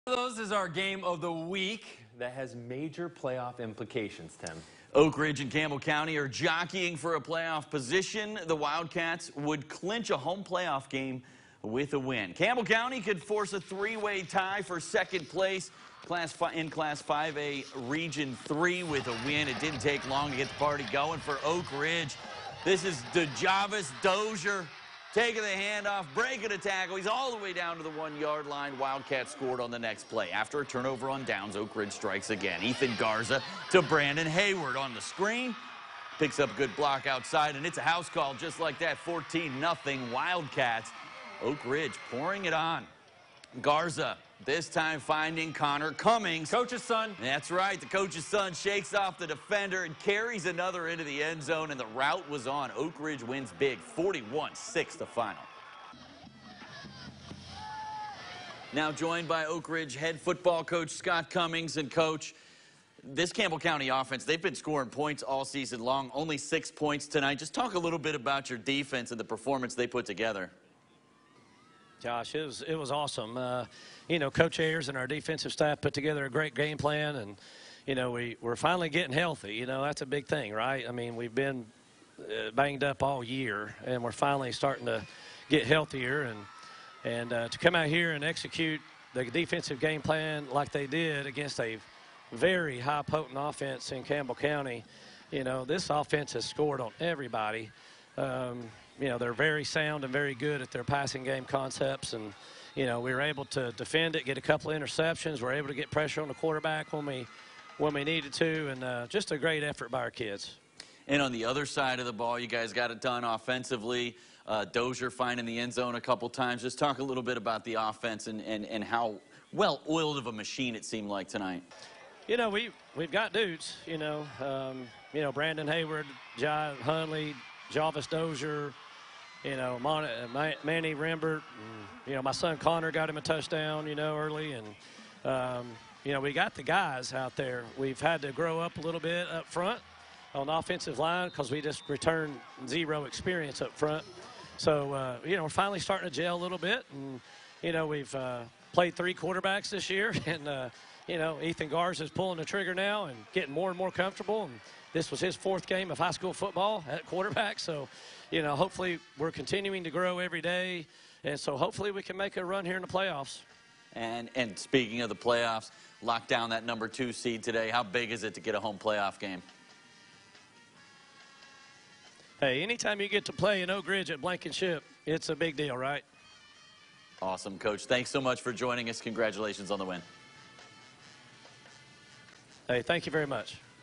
Friday Frenzy Game of the Week: Oak Ridge at Campbell County
East Tennessee High School Football Week 11: Oak Ridge 41 Campbell County 6.